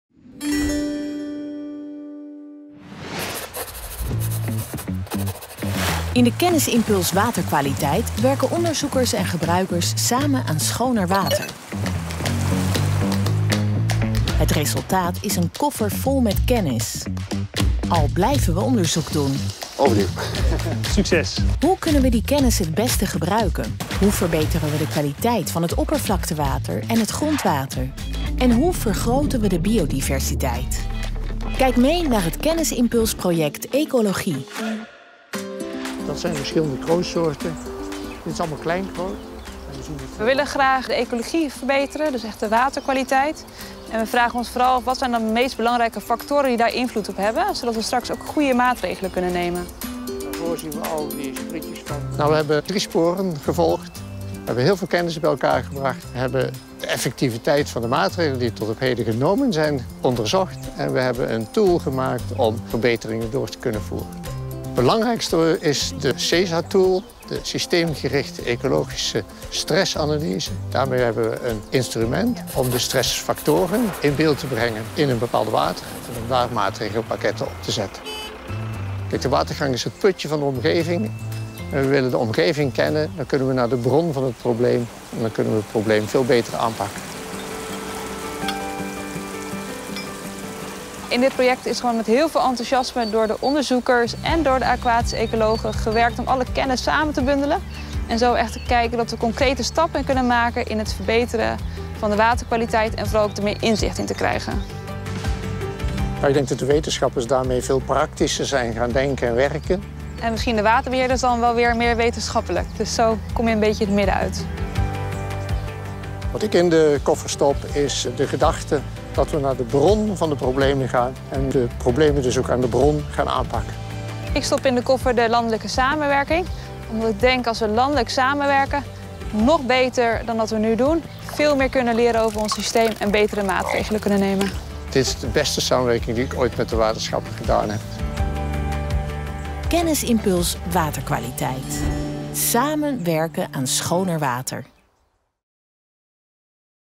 Waterkwaliteit | Kennisimpuls Waterkwaliteit, Ecologie | 2022
In de Kennisimpuls Waterkwaliteit hebben Rijk, provincies, waterschappen, drinkwaterbedrijven en kennisinstituten gezamenlijk gewerkt aan het vergroten van het inzicht in de kwaliteit van het grond- en oppervlaktewater en in de factoren die deze kwaliteit beïnvloeden. Daarmee kunnen waterbeheerders de juiste maatregelen nemen om de waterkwaliteit te verbeteren en de biodiversiteit te vergroten.

In het programma zijn bestaande kennis en inzichten samengebracht, en waar nodig is nieuwe kennis ontwikkeld. Deze kennis is ook (beter) toepasbaar gemaakt voor de praktijk van het waterbeheer, in de vorm van praktisch toepasbare tools en instrumenten. Deze pagina geeft toegang tot alle resultaten van de Kennisimpuls.

Meer weten over de Kennisimpuls Waterkwaliteit? Ga naar